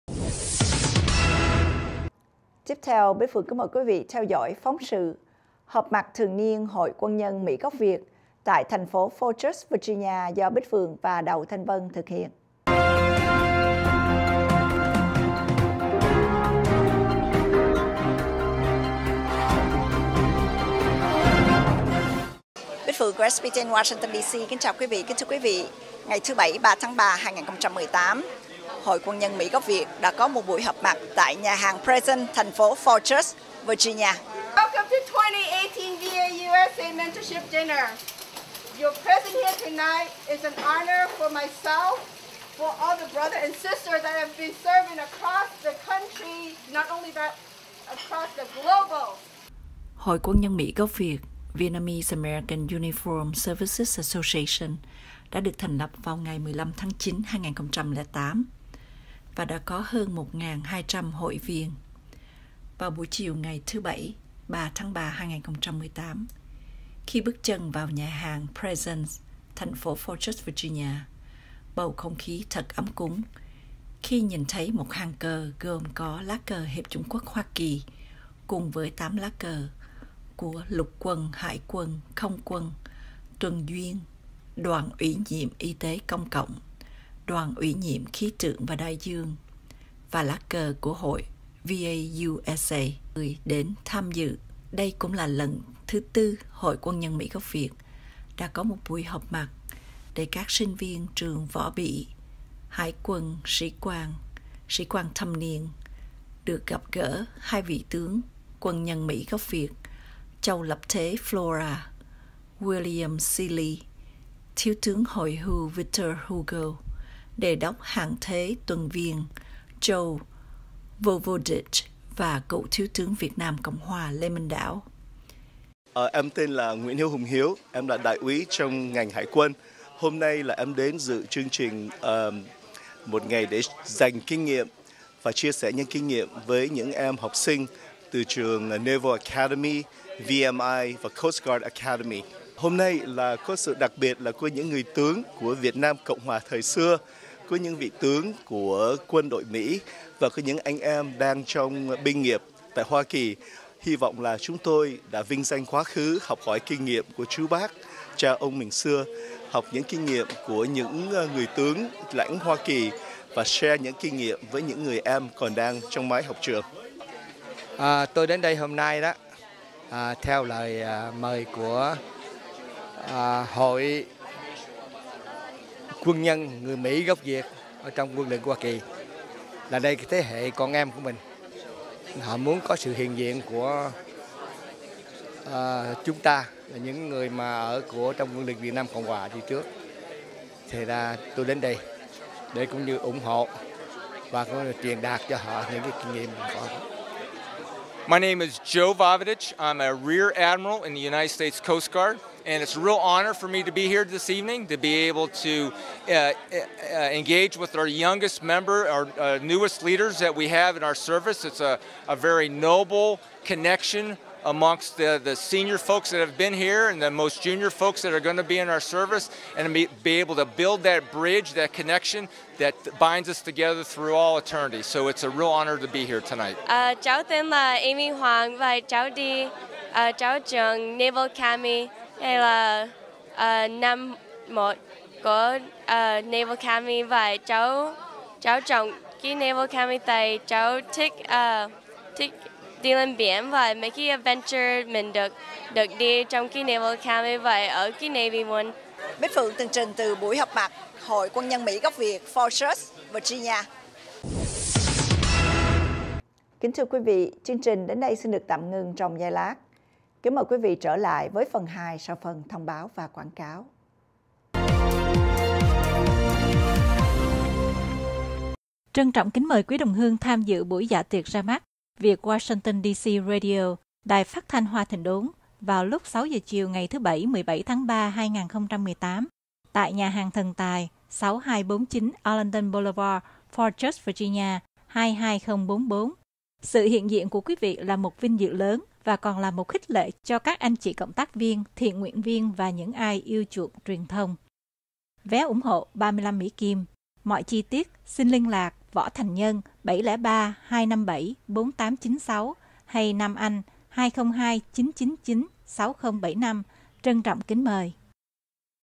Phóng Sự Sinh Hoạt Cộng Đồng: Buổi Họp Mặt Thường Niên Của Hội Quân Nhân Người Mỹ Gốc Việt
SBTN-DC News: Phóng Sự Sinh Hoạt Cộng Đồng: Buổi Họp Mặt Thường Niên Của Hội Quân Nhân Người Mỹ Gốc Việt
06/03/2018
Bích Phượng